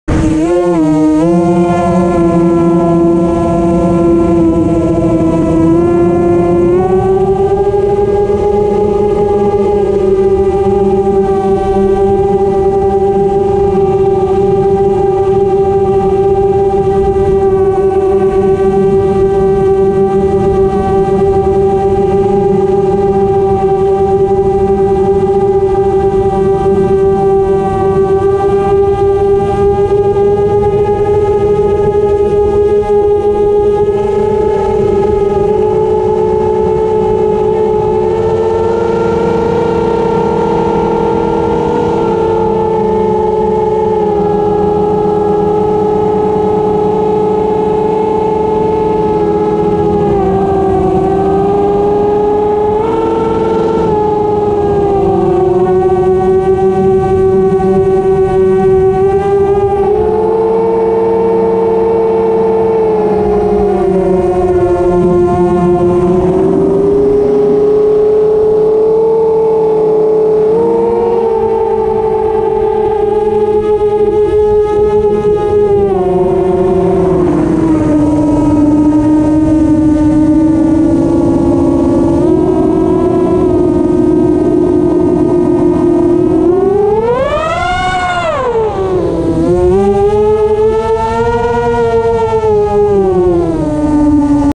Farthest flight?
I think this is the farthest I got away, video got wonky so I turned around.  Antenna placement needs work.  This video cuts off, but I'll post the follow up... It's not so good...

I know I need editing software.  It's an excuse for poor production quality, right?  Speaking of which, these were made with an ND8 on the Session 5, I think it improved things, personally.  Compare to park videos. (Way less jelllloooo)